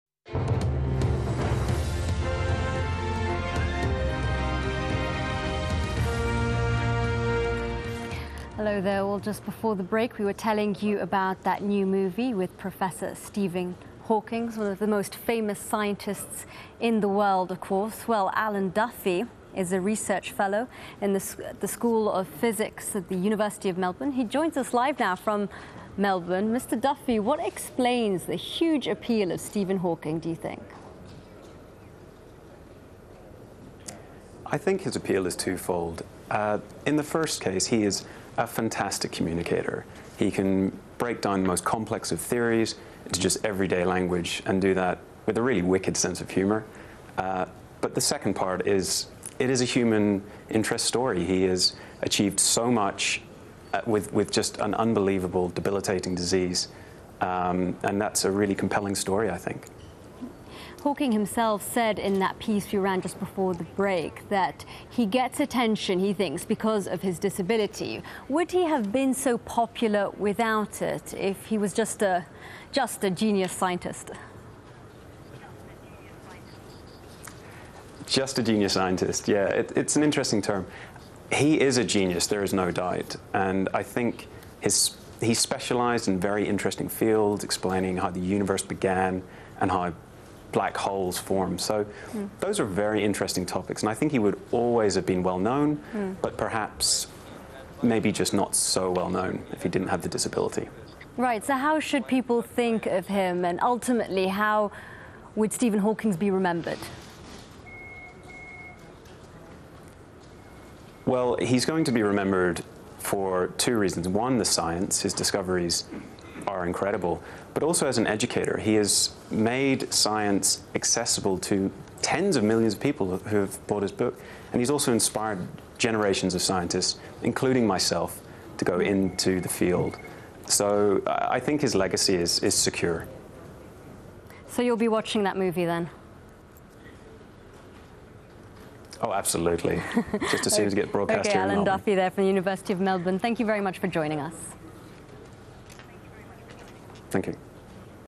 Dr Duffy on Stephen Hawking w Al Jazeera News TV (2013/09/20)
A great opportunity to chat about my hero, Prof Stephen Hawking, with Al Jazeera... My first ever live TV interview and it turned out to be a lot of fun. Sadly I couldn't go into details about his discoveries but did get to acknowledge his influence in my decision to become an astronomer at the University of Melbourne.